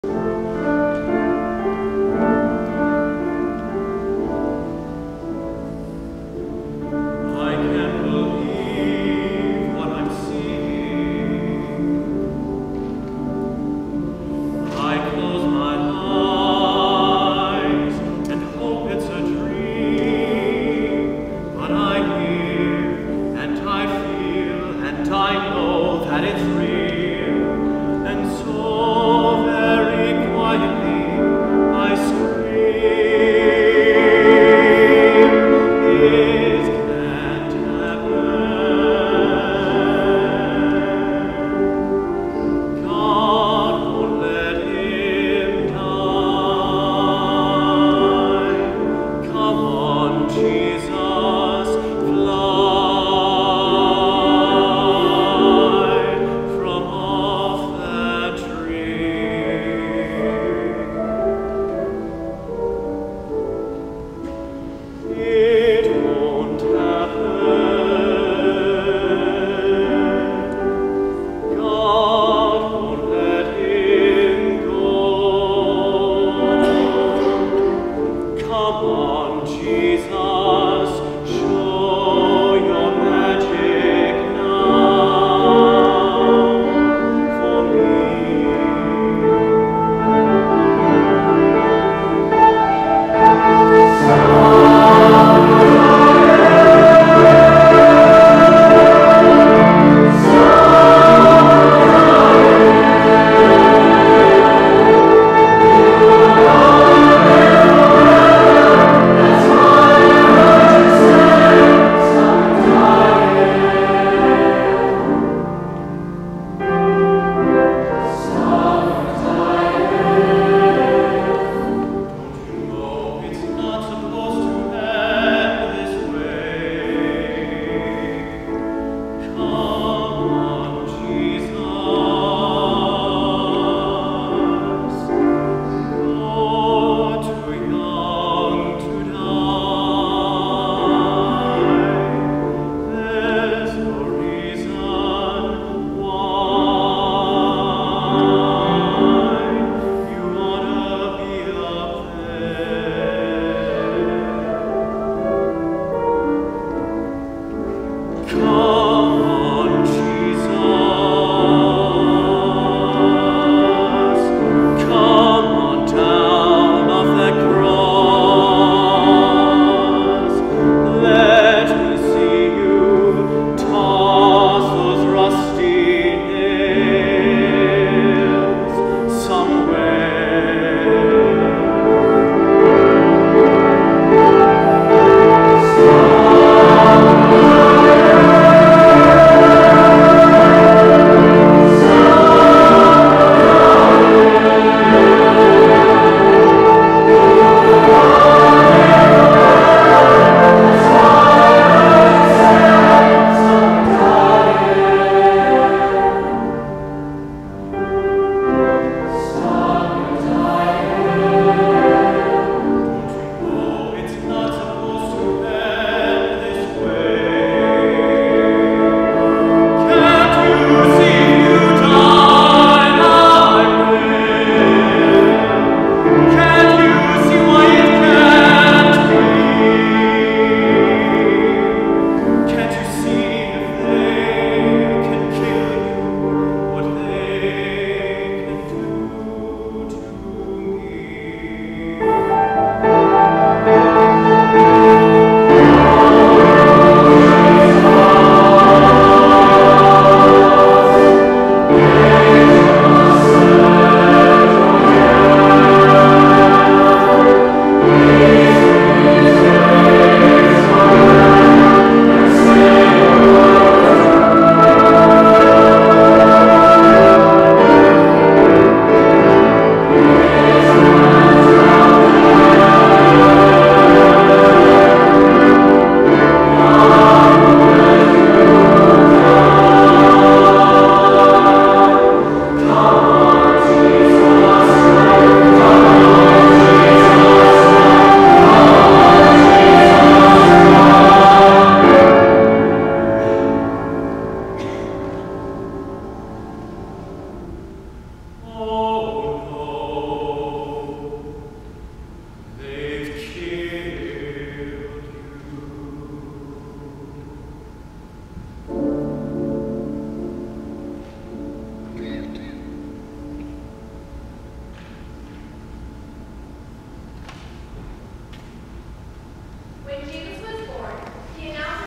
Zach Rude, Solo in the Musical for MASLC, 2016
1. I need to learn how to focus with my new camera when it's on video setting. 2. I need to buy a mic for this camera! Still, it's a great solo. Proud of my boy!